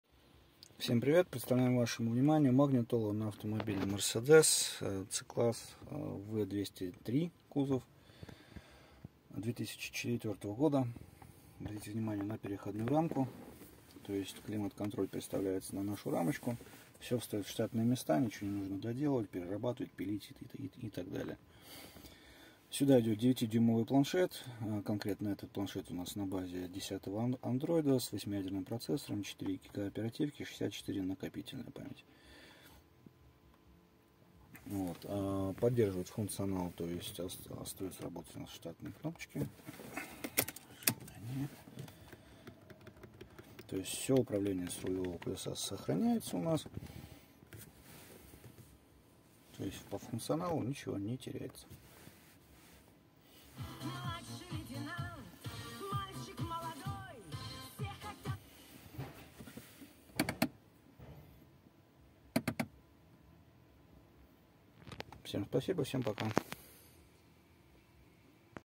MERCEDES C (w203) магнитола UNISON S 2/32 Обзор установки и работы
Центрмагнитол.рф

Штатная магнитола Mercedes C-Class 2 (W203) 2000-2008 UNISON S 2Gb/32Gb, 8 ядер (TG1262)